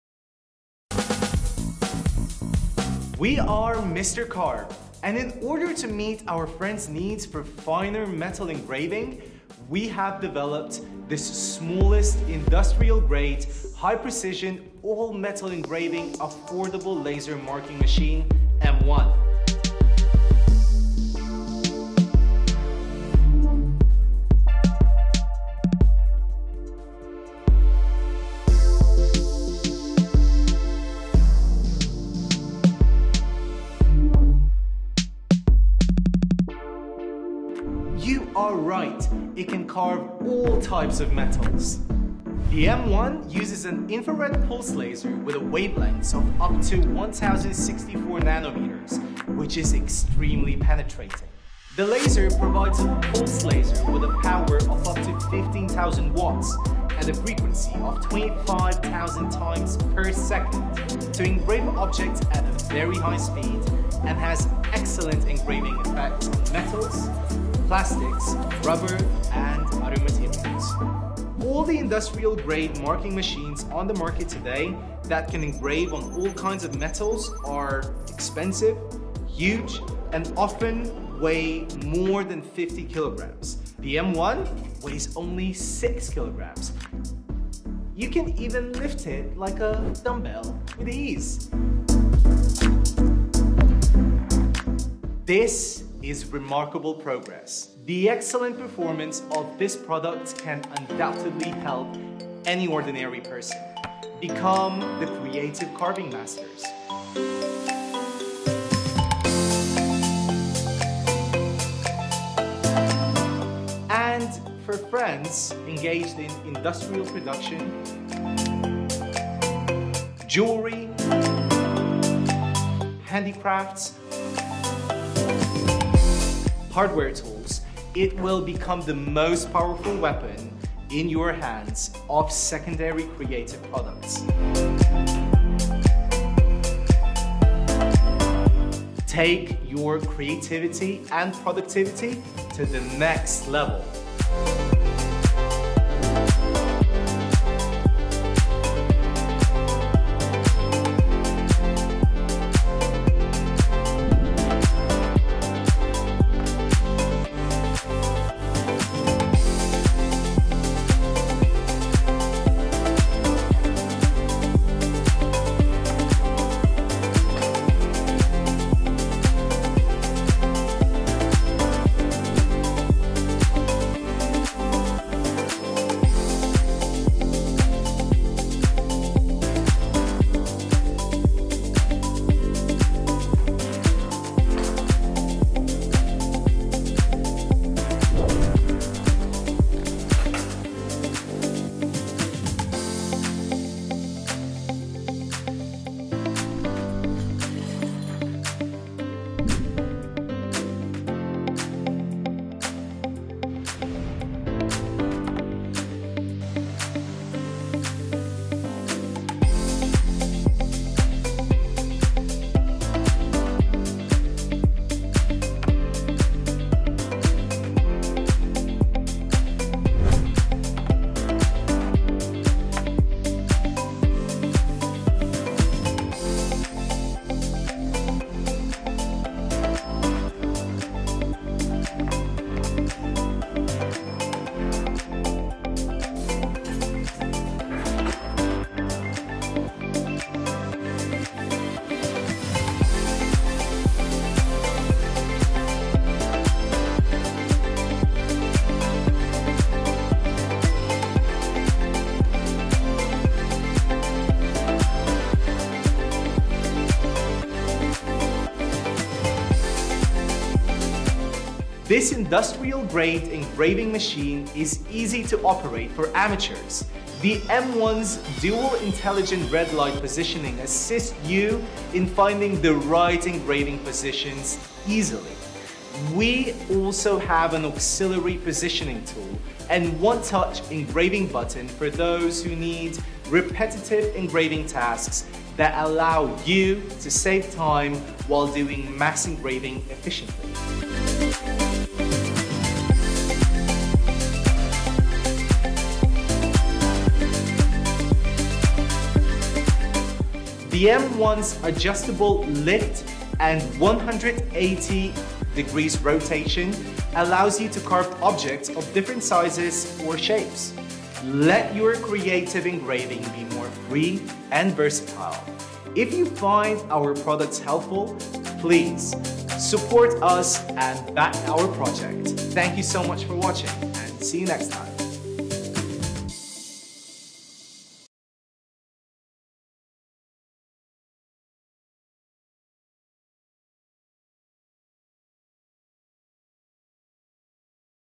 MR. CARVE M1 : MINI ALL-METAL ENGRAVER WITH INDUSTRIAL GRADE QUALITY | Kickstarter | Gizmo-Hub.com
PRE-ORDER THE NEW MR. CARVE M3 (New Campaign Launched May 2022)

Introducing M1, the most compact laser engraver for a diversity of materials, including metal. With industrial-grade resolution, speed, and power, the M1 laser engraver lets you engrave clearly and quickly on a variety of surfaces, from metal to plastic, and even leather. Use it for personal items or business projects.  Either way, it’s an amazingly easy and handy tool to have around.